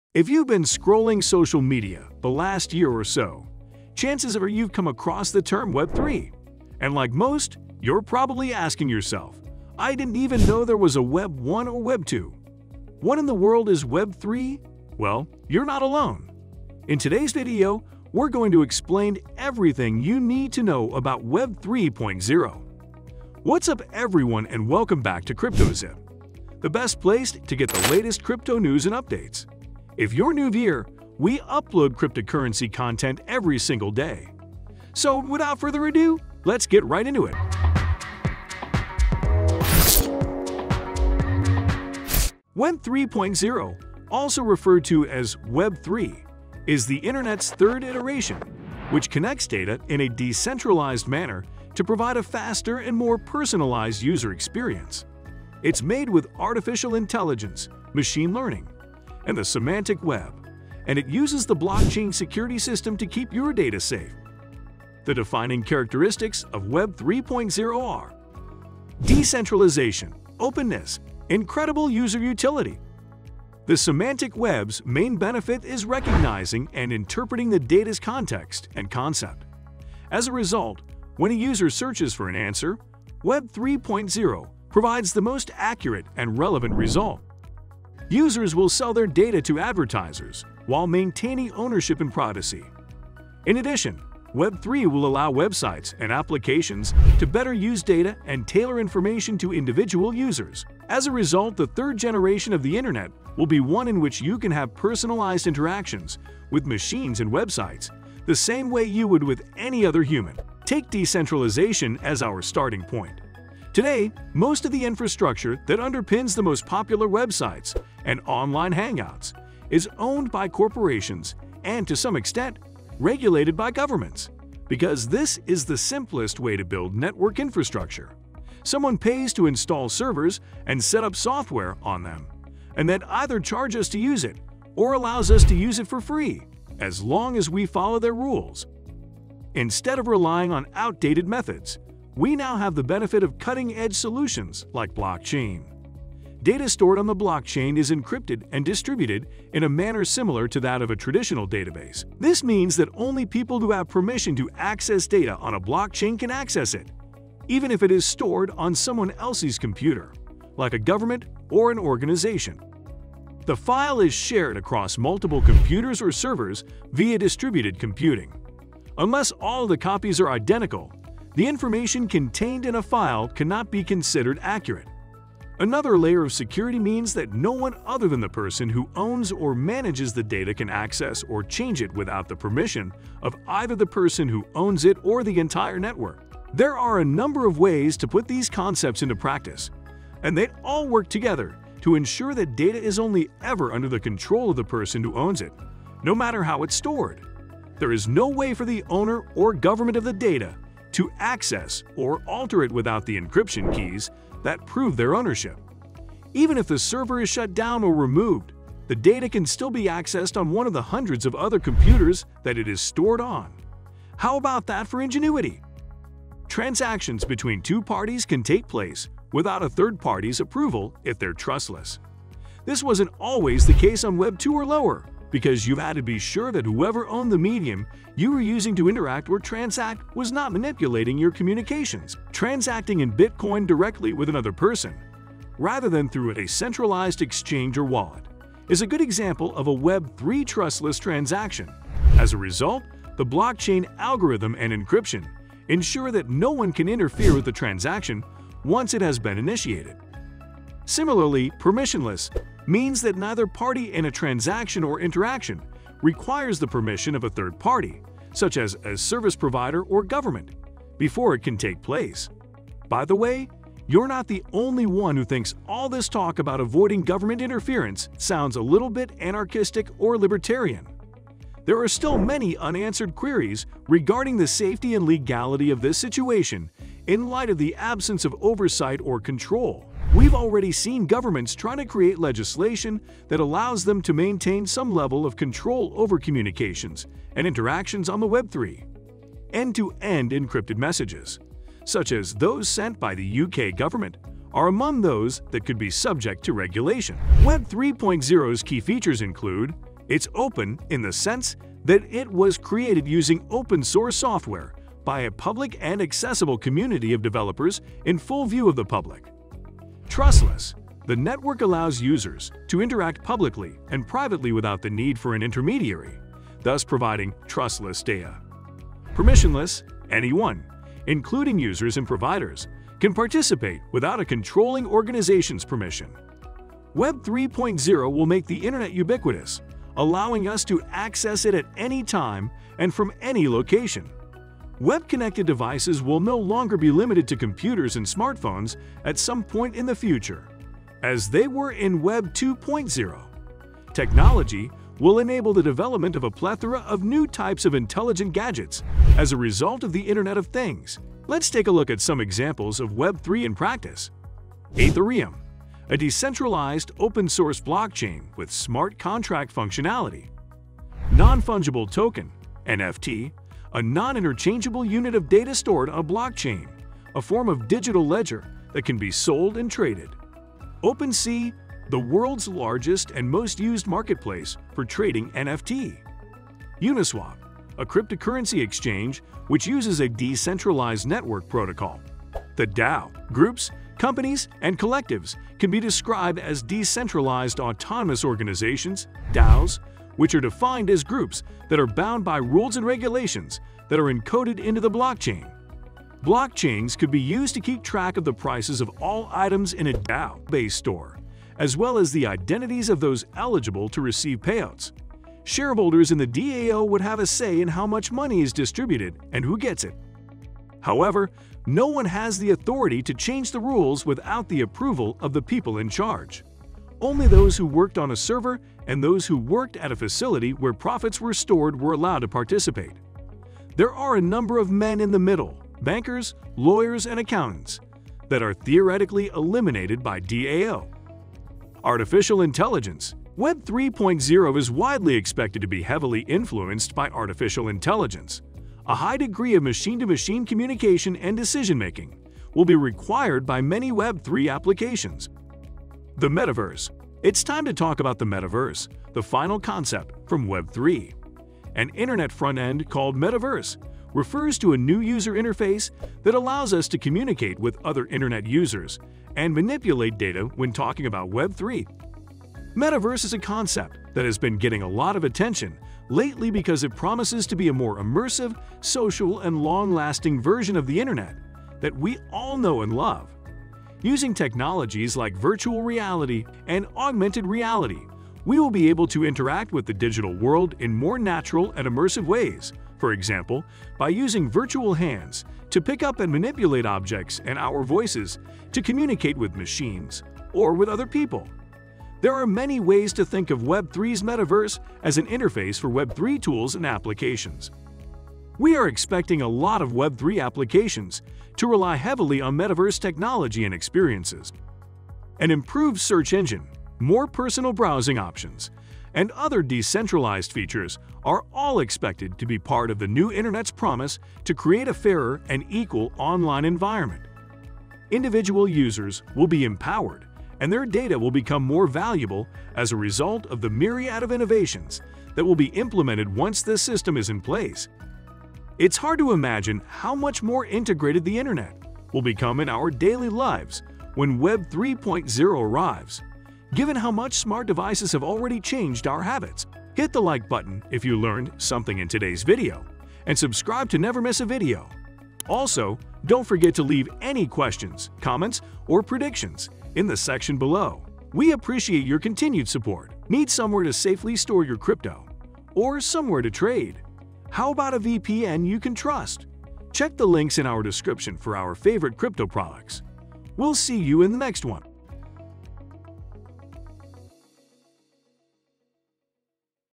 What Is Web3? | Web 3.0 Explained!
If you’ve been scrolling social media the last year or so, chances are you’ve come across the term Web 3. And like most, you’re probably asking yourself, I didn’t even know there was a web 1 or web 2? What in the world is web 3? Well, you’re not alone. In today’s video, we’re going to explained what everything you need to know about Web 3.0. What Is Web3? | Web 3.0 Explained.

Web 3.0 (also referred to as web3) is the Internet's third iteration, which connects data in a decentralized manner to provide a faster and more personalized user experience. It's made with artificial intelligence, machine learning, and the semantic web, and it uses the blockchain security system to keep your data safe.

0:00 Intro
0:42 Web 3.0 Explained
1:50 What Is Decentralization
3:08 Web 3.0 Utility
4:56 Web 3.0 Key Features
5:57 Web 3.0 Examples
6:25 Future Of Web 3.0
8:25 Final Thoughts

⬇️ Watch Our Latest Cryptocurrency videos!⬇️

What Is Ripple XRP? | XRP Explained!

CryptoZip